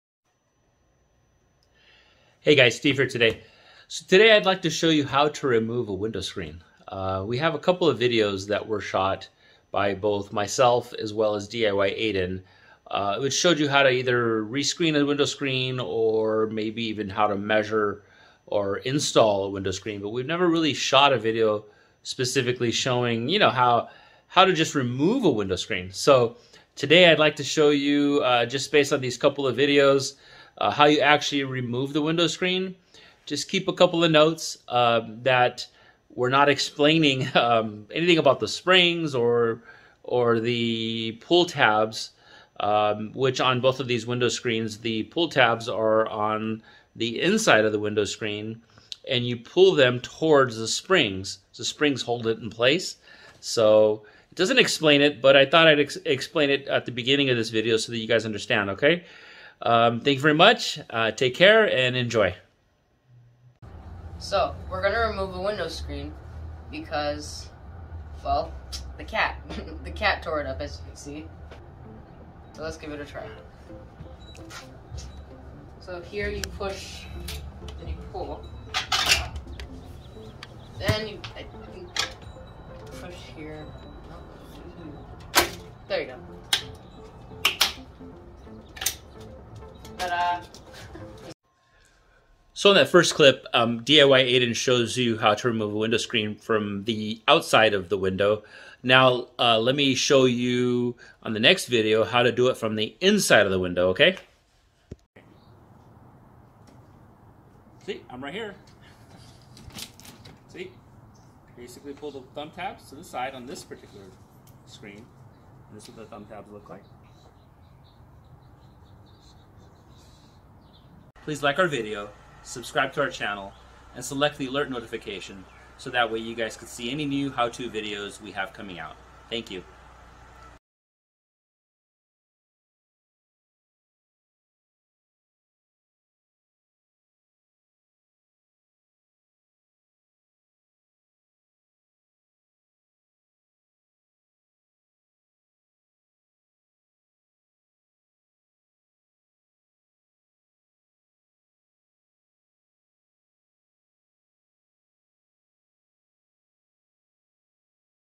How to take a screen off a window
Steve Tristan & DIY Aidan shows us how to easily remove a screen from a window. Window Screens can sometimes be tricky. Especially in vinyl windows. Why? Well it's because they make vinyl windows less expensive and use all vinyl or plastic parts. Except for the window screen frame, that's aluminum. The metal on these types of window tends to be thinner then most and removing them incorrectly could lead to damaging the window screen. Please watch our video to see how to do it the right way.

Taking a Screen Off a Window: A Step by Step Video Tutorial

Not sure how to take the screen off a window? This step by step video tutorial will show you everything you need to know!

Taking a screen off a window may seem daunting, but with the right supplies and step-by-step instructions, it can be a simple process. This video tutorial will show you how to remove the screen from your window safely and efficiently.

To remove a screen from a vinyl window, follow these steps:Locate the window screens tension springs and pull tabs. Tension springs are usually a shiny piece of metal that is on the side or the top of the window screen.
The pull tabs can be back, grey or clear and are usually made of plastic. If the tension springs are tight and its difficult to remove the screen, then use a flat head screwdriver or putty knife to gently pry the screen out. Once the  window screen frame is freed from the window channel then removing it should be easy to take off.

To reattach the screen, simply reverse the process by fitting the screen back into the window frame and clicking it back into place. Make sure the screen is properly aligned and that it’s entirely inside of all four sides of the window channel.

If you are having difficulty removing the screen or if the retention devices are damaged, it may be best to consult with a professional or the manufacturer for assistance.It's important to handle the screen frames and mesh gently to avoid damaging them or the vinyl window frame. Use caution when using tools to avoid scratching or damaging the window.